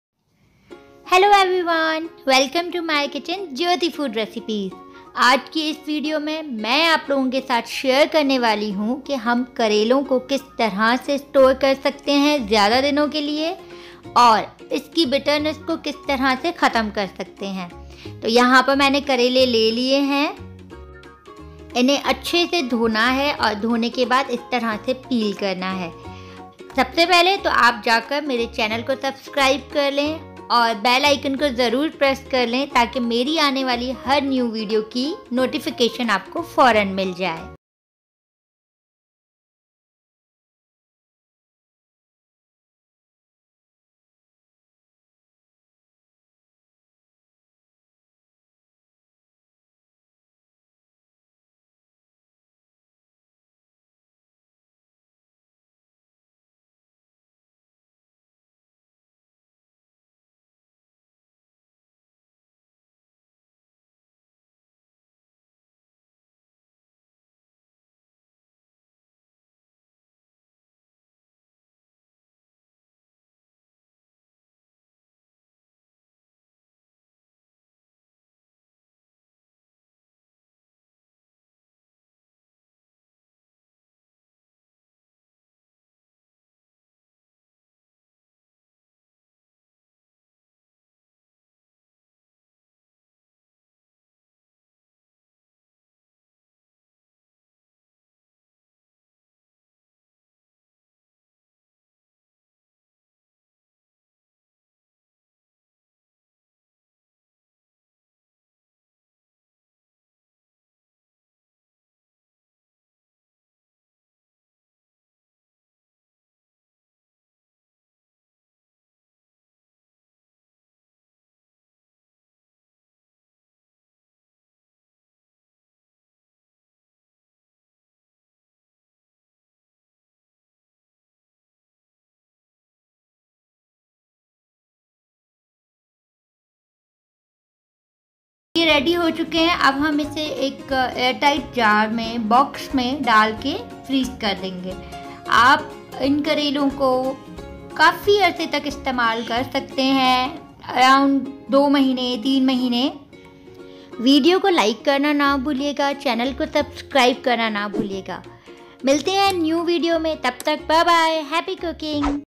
How To Preserve Bitter Guard For A Long Time | karele Store Krny Ka Tariqa | Tips And Tricks
in this video, we are going to tell you that how do we store bitter gourd/ karele for a long time. easy and unique tips and tricks that you must apply. in this method you can store bitter gourd \ karele for around 2 to 3 months. i will show you how to make bitter gourd curry on my next upload

here are my some latest videos links